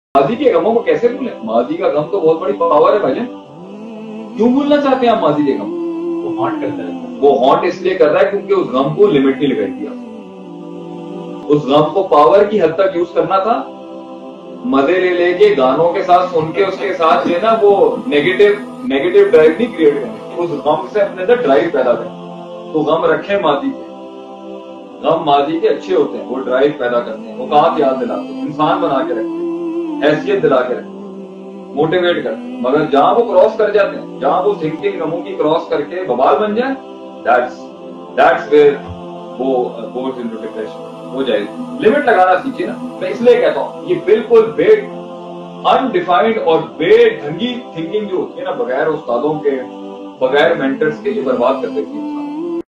Maazi Kay Gham Ko Kese Bhoolen? || Shaykh Atif Ahmed || Al Midrar Institute
سائل کا سوال کے ماضی کے غموں سے کیسے نکلا جائے اس پر ایک مختصر جواب!!